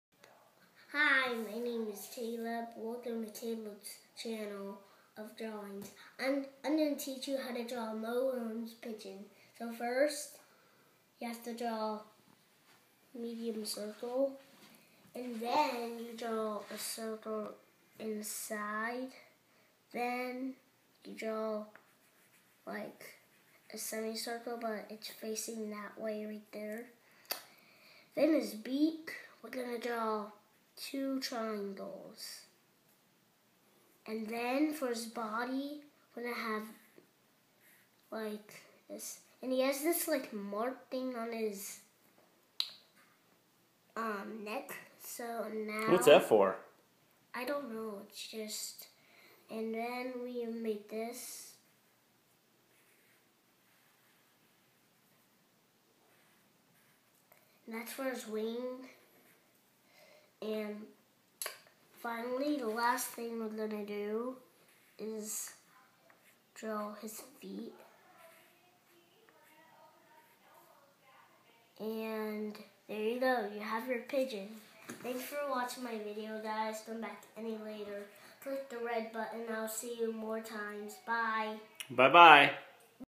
Caleb Draws a Pigeon
Caleb shows how to draw a pigeon.